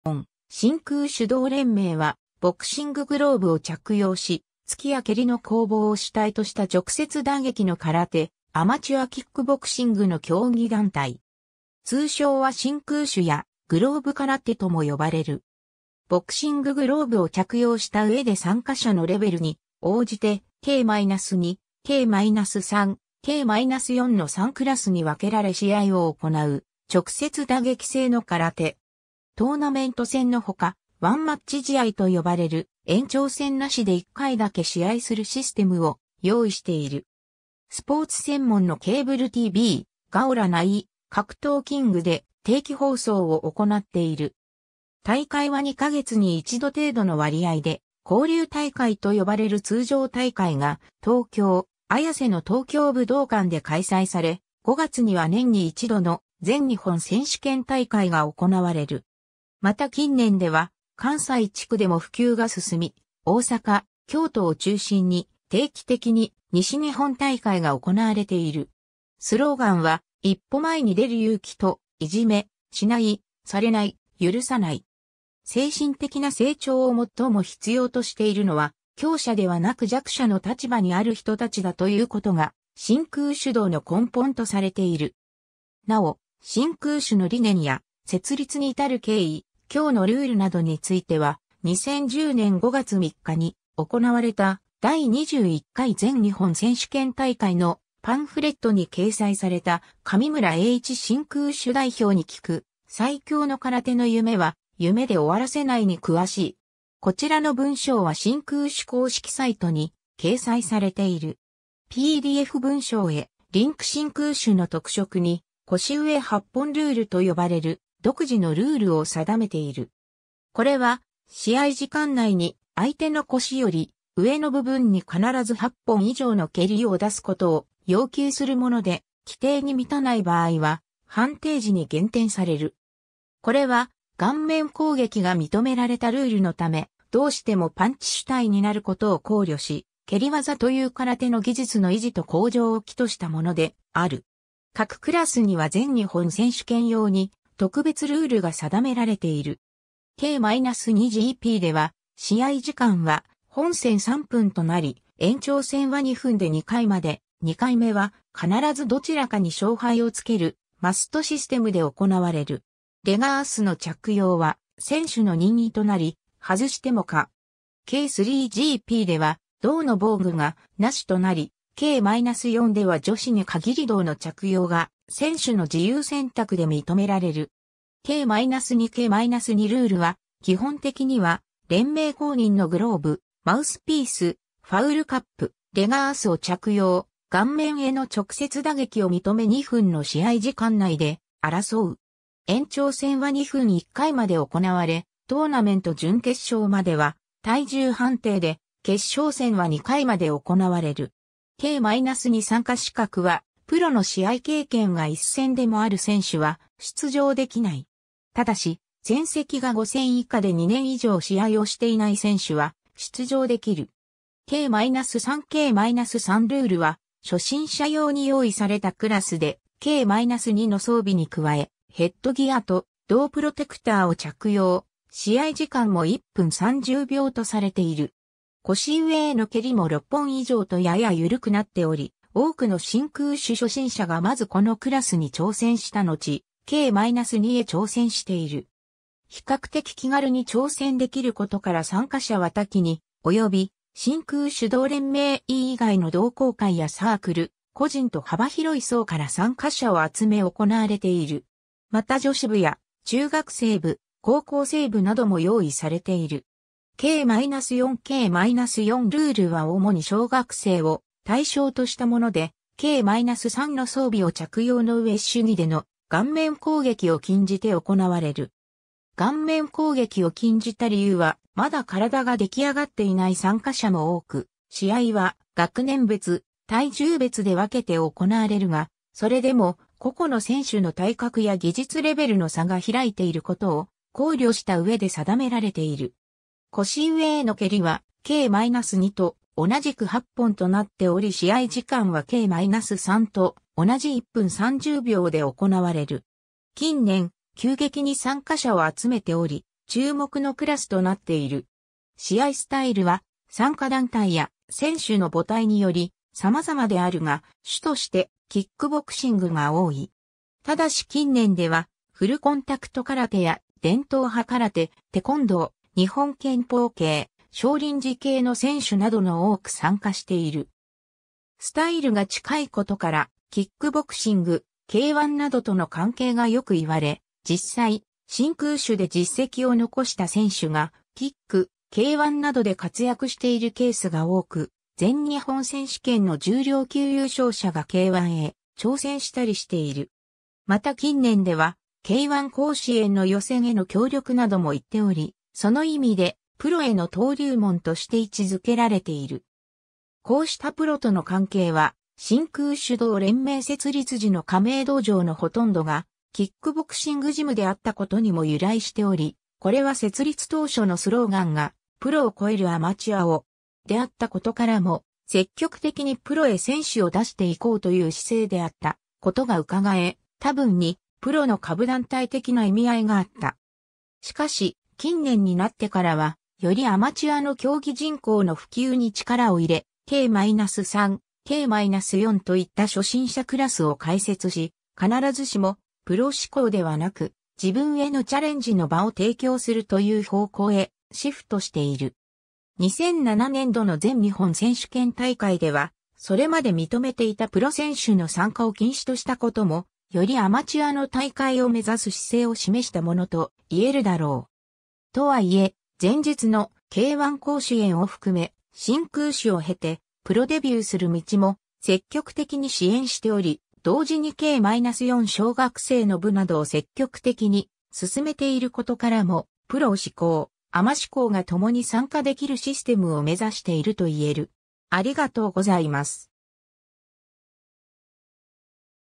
全日本新空手道連盟
特定非営利活動法人_(スポーツ関係)
新空手（グローブ空手）の試合 NPO法人全日本新空手道連盟 （ぜんにほんしんからてどうれんめい）は、ボクシンググローブを着用し、突きや蹴りの攻防を主体とした直接打撃の空手・アマチュアキックボクシングの競技団体。
通称は新空手やグローブ空手とも呼ばれる。
 ボクシンググローブを着用した上で参加者のレベルに応じてK-2、K-3、K-4の3クラスに分けられ試合を行う直接打撃制の空手。
トーナメント戦の他、ワンマッチ試合と呼ばれる延長戦なしで1回だけ試合するシステムを用意している。
スポーツ専門のケーブルTV「GAORA」内「格闘KING」で定期放送を行っている。
大会は2か月に一度程度の割合で「交流大会」と呼ばれる通常大会が東京・綾瀬の東京武道館で開催され、5月には年に一度の全日本選手権大会（通称Grand Prix）が行われる。
また近年では、関西地区でも普及が進み、大阪・京都を中心に定期的に西日本大会が行われている。
 スローガンは「一歩前に出る勇気」と「いじめ、しない、されない、許さない」。
「精神的な成長を最も必要としているのは、強者ではなく弱者の立場にある人達だ」と言うことが新空手道の根本とされている。
 なお、新空手の理念や、設立に至る経緯、今日のルールなどについては、2010年5月3日に行われた「第21回全日本選手権大会」のパンフレットに掲載された「神村榮一新空手代表に聞く、最強の空手の夢は、夢で終わらせない」に詳しい。
PDF文章へリンク 新空手の特色に「腰上8本ルール」と呼ばれる独自のルールを定めている。
これは試合時間内に相手の腰（帯）より上の部分に必ず8本以上の蹴りを出すことを要求するもので、規定に満たない場合は判定時に減点される。
これは顔面攻撃が認められたルールのため、どうしてもパンチ主体になることを考慮し、「蹴り技」という空手の技術の維持と向上を企図したものである。
 各クラスには全日本選手権用に特別ルールが定められている。
K-2GPでは試合時間は本戦3分となり、延長戦は2分で2回まで、2回目は必ずどちらかに勝敗をつけるマストシステムで行われる。
レガースの着用は選手の任意となり、外しても可。
K-3GPでは胴の防具がなしとなり、K-4では女子に限り胴の着用が選手の自由選択で認められる。
 K-2 K-2ルールは基本的には連盟公認のグローブ、マウスピース、ファウルカップ、レガース（脛パット）を着用、顔面への直接打撃を認め2分の試合時間内で争う。
延長戦は2分1回まで行われ、トーナメント準決勝までは体重判定で、決勝戦は2回まで行われる。
K-2参加資格はプロの試合経験が1戦でもある選手は出場できない。
ただし、戦績が5戦以下で2年以上試合をしていない選手は出場できる。
 K-3 K-3ルールは初心者用に用意されたクラスで、K-2の装備に加えヘッドギアと胴プロテクターを着用、試合時間も1分30秒とされている。
腰上への蹴りも6本以上とやや緩くなっており、多くの新空手初心者がまずこのクラスに挑戦したのちK-2へ挑戦している。
比較的気軽に挑戦できることから参加者は多岐に及び、新空手道連盟以外の同好会やサークル、個人と幅広い層から参加者を集め行われている。
また女子部や中学生部、高校生部なども用意されている。
 K-4 K-4ルールは主に小学生を対象としたもので、K-3の装備を着用のうえ手技での顔面攻撃を禁じて行われる。
顔面攻撃を禁じた理由は、まだ身体ができあがっていない参加者も多く、試合は学年別、体重別で分けて行われるが、それでも個々の選手の体格や技術レベルの差が開いていることを考慮したうえで定められている。
腰上への蹴りはK-2と同じく8本となっており試合時間はK-3と同じ1分30秒で行われる。
近年、急激に参加者を集めており注目のクラスとなっている。
 試合スタイルは参加団体や選手の母体により様々であるが、主としてキックボクシングが多い。
ただし近年ではフルコンタクト空手や伝統派空手、テコンドー、日本拳法系、少林寺系の選手などの多く参加している。
 スタイルが近いことからキックボクシング、K-1などとの関係がよく言われ、実際、新空手で実績を残した選手がキック、K-1などで活躍しているケースが多く、全日本選手権の重量級優勝者がK-1へ挑戦したりしている。
また近年では「K-1甲子園...